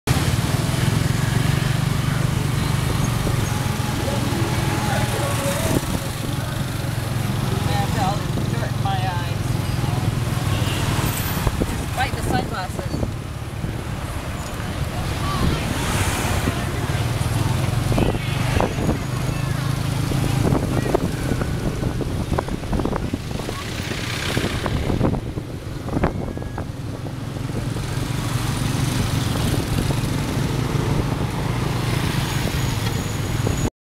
Tuk Tuk ride through Siem Reap, Cambodia.....home of Angkor Wat
Enjoy a tuk tuk ride on the busy streets of Siemp Riep. See how many people can fit on a motorbike!